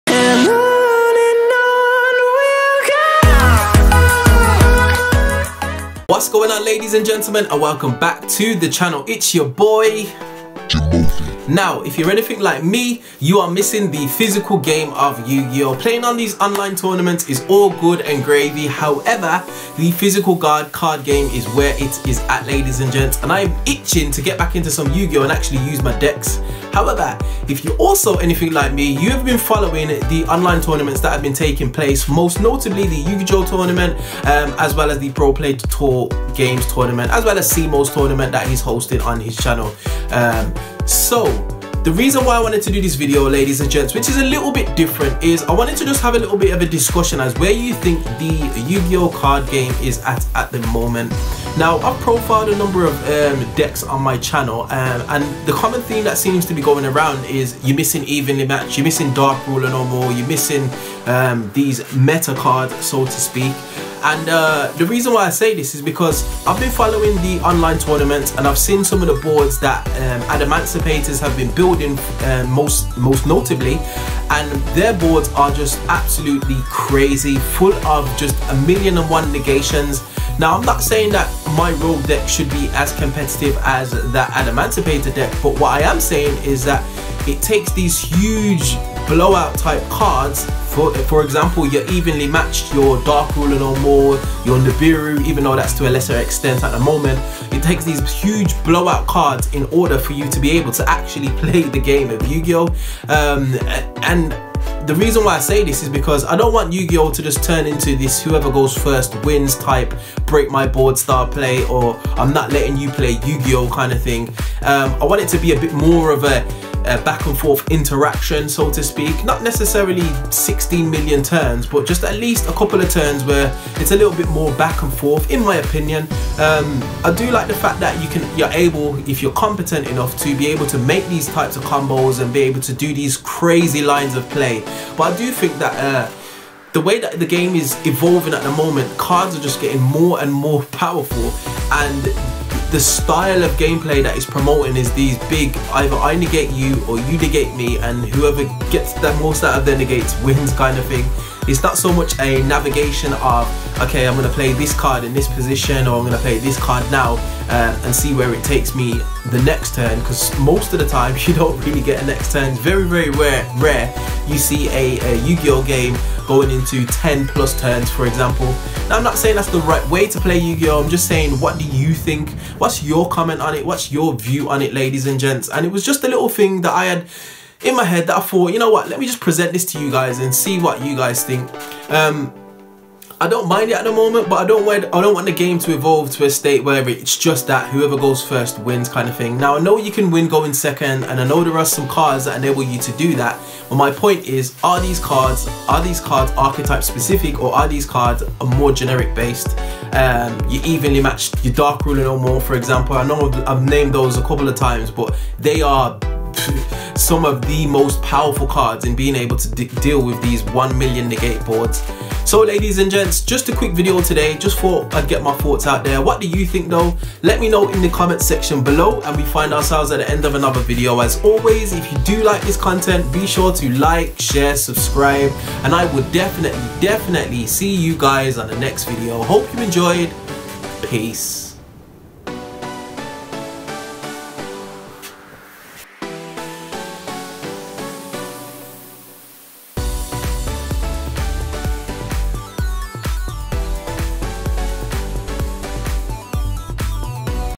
Yu-Gi-Oh! State of the Game (TCG) Short Discussion (May 2020)
Just a short video discussing the state of the game and this brake my board style game play that seems to be ever more present in the modern game of Yu-Gi-Oh! What are your thoughts?